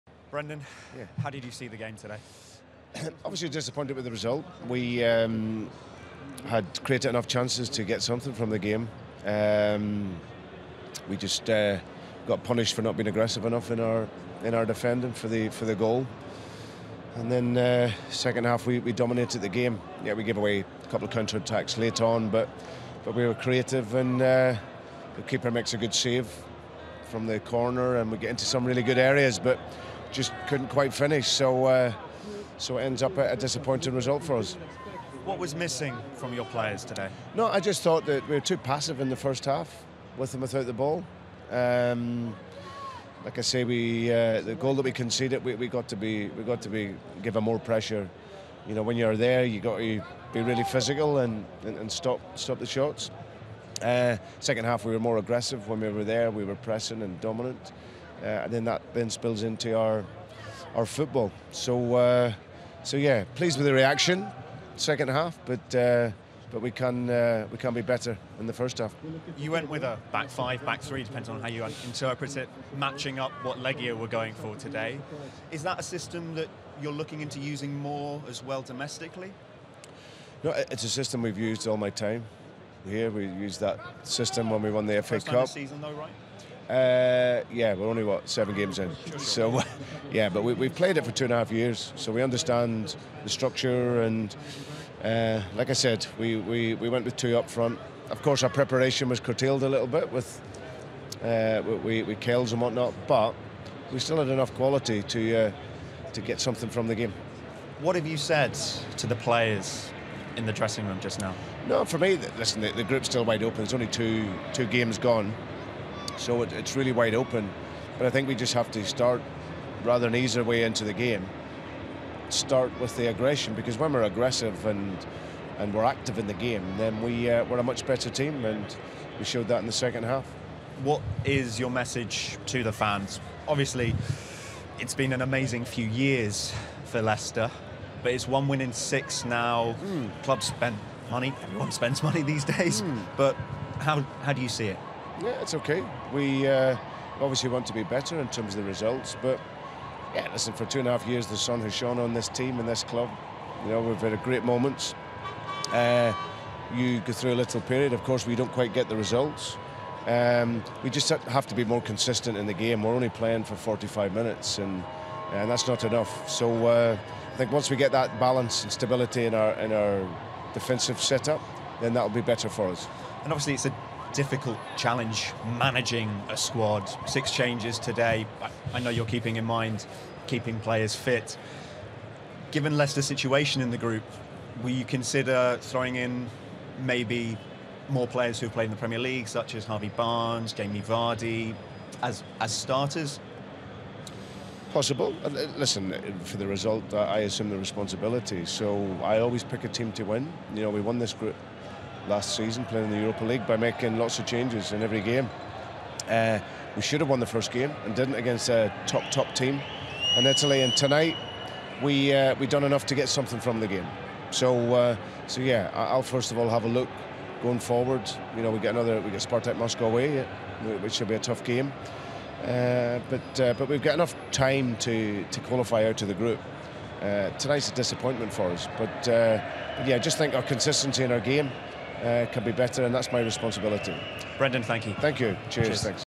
"The group is still wide open. Only two games have gone." Brendan Rodgers on Foxes' tough UEL start
Brendan Rodgers reflects on Leicester's defeat against Legia and a tough start to their Europa League campaign...